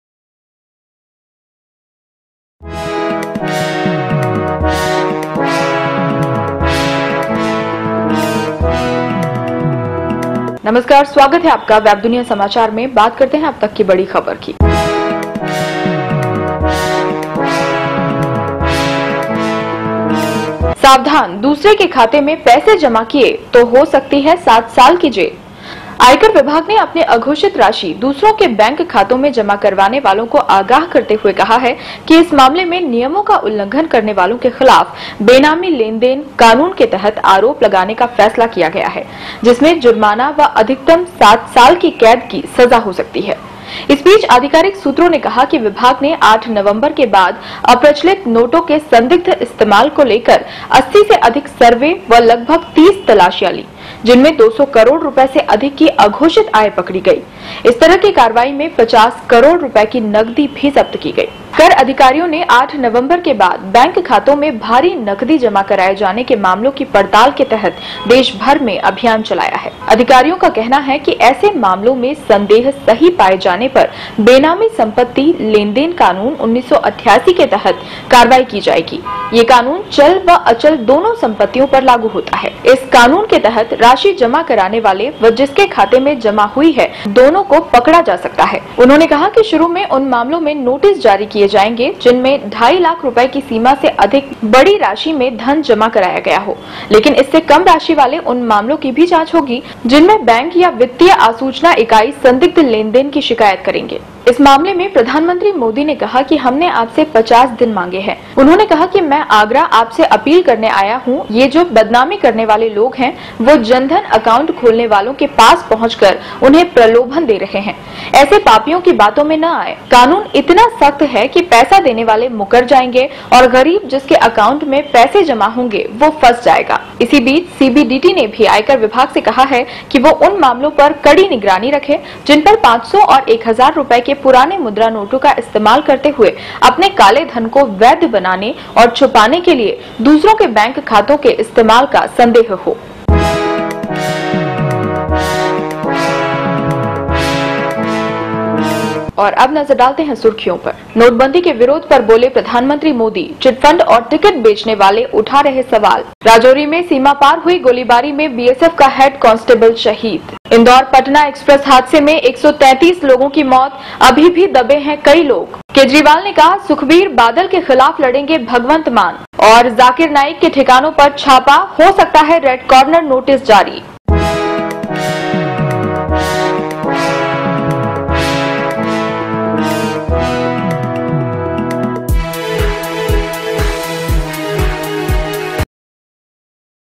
दूसरे के खाते में पैसे जमा किए तो 7 साल की जेल | Bank account violators will face 7 years jail
आयकर विभाग ने अपनी अघोषित राशि दूसरों के बैंक खातों में जमा करवाने वालों को आगाह करते हुए कहा है कि इस मामले में नियमों का उल्लंघन करने वालों के खिलाफ बेनामी लेन-देन कानून के तहत आरोप लगाने का फैसला किया गया है जिसमें जुर्माना व अधिकतम 7 साल की कैद की सजा हो सकती है।

इस बीच आधिकारिक सूत्रों ने कहा है कि विभाग ने 8 नवंबर के बाद अप्रचलित नोटों के संदिग्ध इस्तेमाल को लेकर 80 से अधिक सर्वे व लगभग 30 तलाशियां लीं जिनमें 200 करोड़ रुपए से अधिक की अघोषित आय पकड़ी गई। इस तरह की कार्रवाई में 50 करोड़ रुपए की नकदी भी जब्त की गई है। कर अधिकारियों ने 8 नवंबर के बाद बैंक खातों में भारी नकदी जमा कराए जाने के मामलों की पड़ताल के तहत देशभर में अभियान चलाया है।

अधिकारियों का कहना है कि ऐसे मामलों में संदेह सही पाए जाने पर बेनामी संपत्ति लेन-देन कानून 1988 के तहत कार्रवाई की जाएगी। यह कानून चल व अचल दोनों संपत्तियों पर लागू होता है। इस कानून के तहत राशि जमा कराने वाले व जिसके खाते में जमा हुई, दोनों को पकड़ा जा सकता है। उन्होंने कहा कि शुरू में उन मामलों में नोटिस जारी किए जाएंगे जिनमें 2.50 लाख रुपए की सीमा से अधिक बड़ी राशि में धन जमा कराया गया हो, लेकिन इससे कम राशि वाले उन मामलों की भी जांच होगी जिनमें बैंक या वित्तीय आसूचना इकाई संदिग्ध लेन-देन की शिकायत करेंगे।